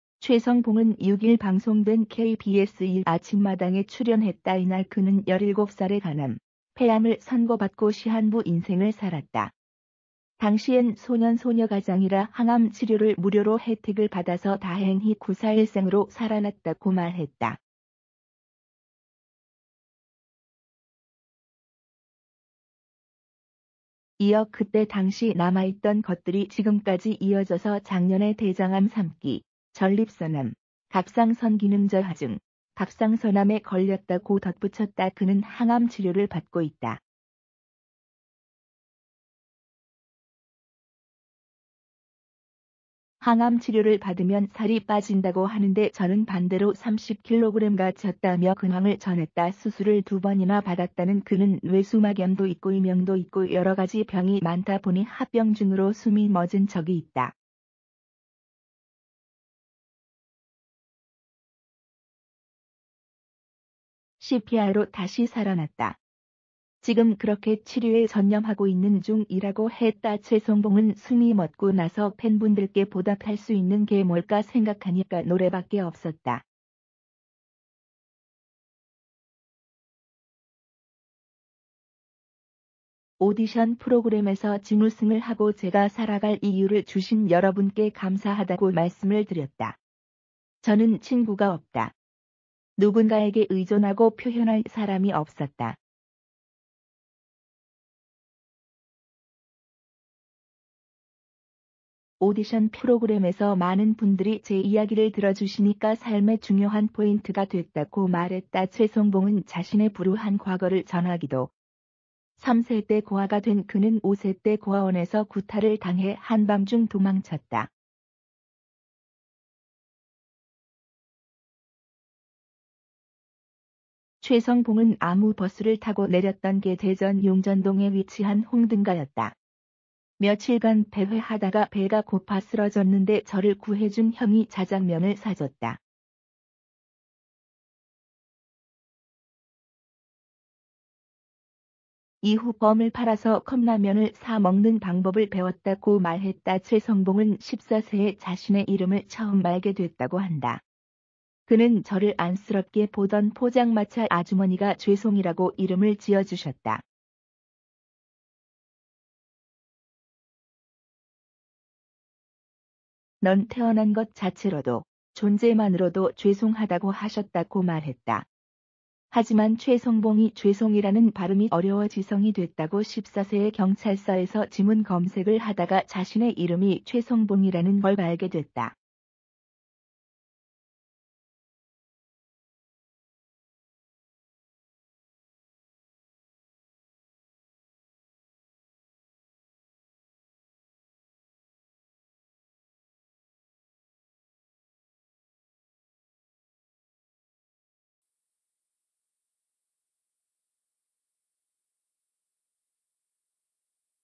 가수 최성봉 “대장암·전립선암·갑상선암 판정”… 근황 보니
가수 최성봉